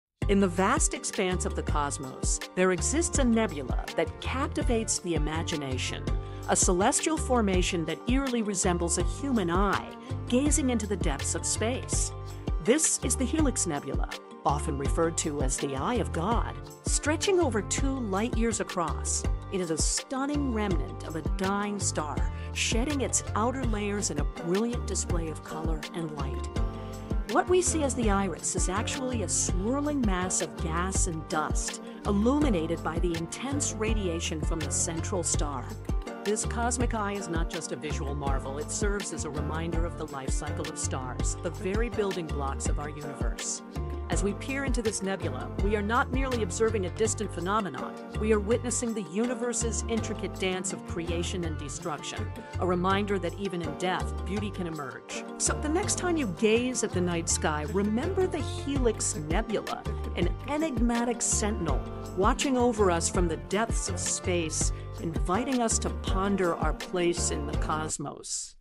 This Nebula Looks Like a Human Eye Watching You 👁️🌌CosmicEye
Is the universe watching us back? 👁️🌌 This eerie nebula resembles a giant human eye gazing across the cosmos — and it’s both beautiful and unsettling. Captured in stunning detail, this celestial formation blurs the line between science and the supernatural. A jaw-dropping reminder that space is full of wonders we still don’t understand. Are we truly alone… or being watched from the stars?